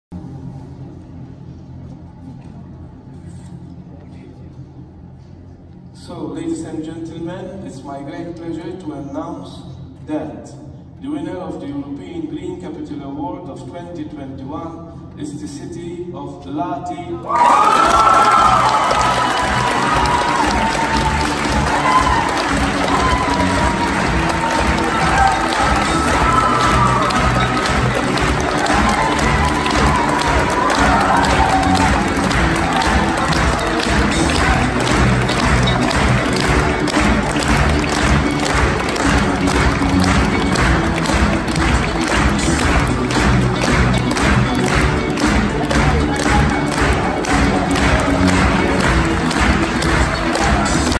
Lahti (Finlandia), elegida como Capital Verde Europea a partir del año 2021
La ciudad finlandesa de Lahti ha sido elegida por la Comisión Europea como la próxima Capital Verde Europea a partir del año 2021, con lo que sucederá a Lisboa, merecedora del título el próximo año, y a Oslo, ciudad que ostenta en la actualidad el galardón y anfitriona de la ceremonia. Con esta decisión, las ciudades francesas de Lille y Estrasburgo se han quedado a las puertas, resultando finalistas.